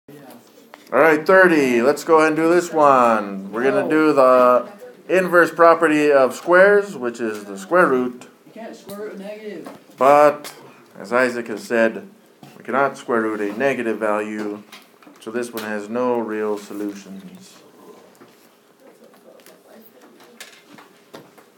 Solve Equations with Squares: m^2= -81
The Problem: Solve each equation.
m^2= -81

The Solution: no real solutions